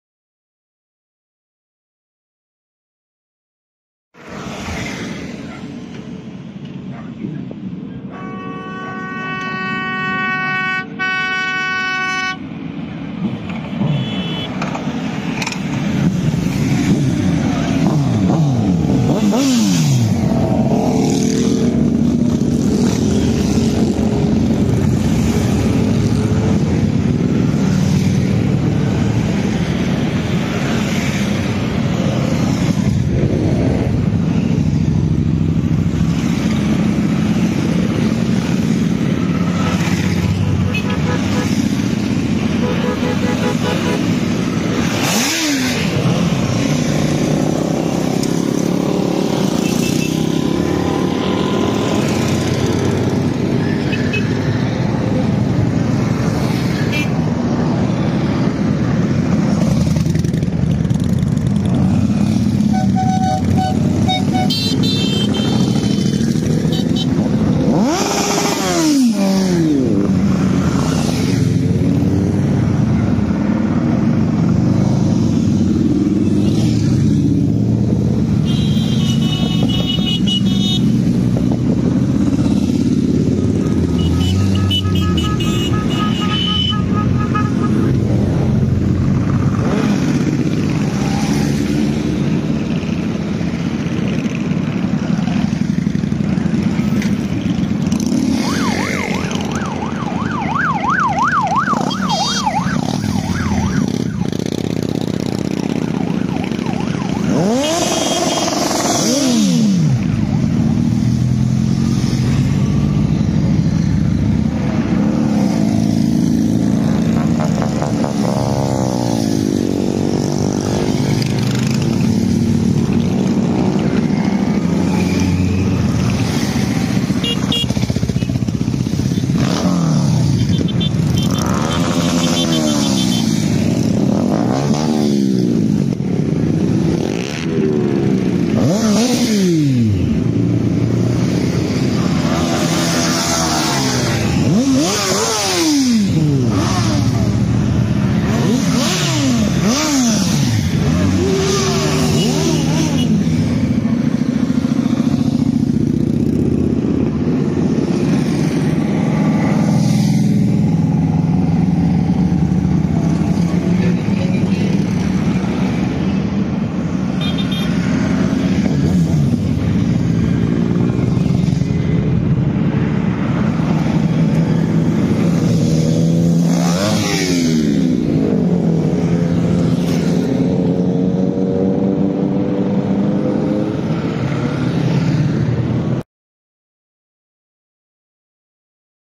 5 motocyklowe Powitanie Wiosny z MGzk - Przejazd do Ślesina
A tak jechaliśmy do miejsca gdzie Marzanna dokonała swojego żywota :)

Tutaj ok.  170 maszyn.. 💪💪
Tutaj warto wspomnieć , że przynajmniej 50 motocykli czekało już na resztę kolumny w Ślesinie.
Jadąc wspólnie w Koninie, zatrzymali się na "czerwonym" i odłączyli się od głównej kolumny,  jadąc później  bezpośrednio do Mariny w Ślesinie...